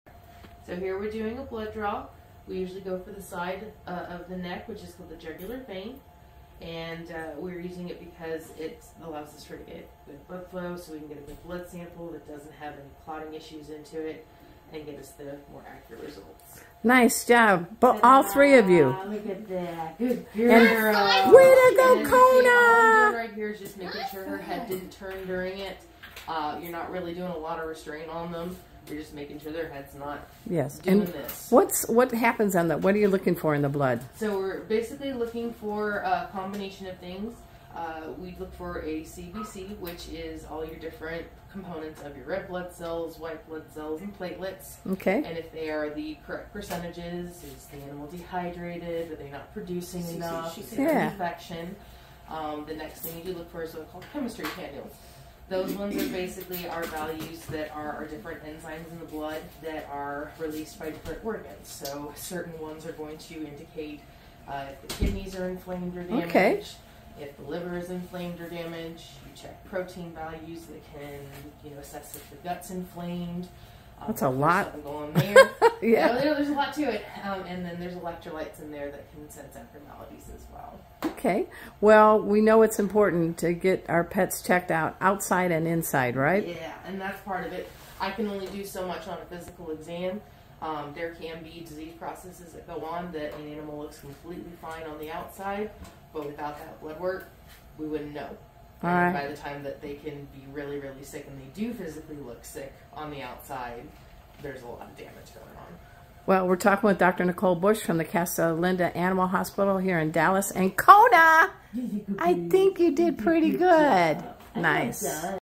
Kona Exam - Blood Draw
Pet Safety Dog Kona gets her annual exam! Time for the blood draw.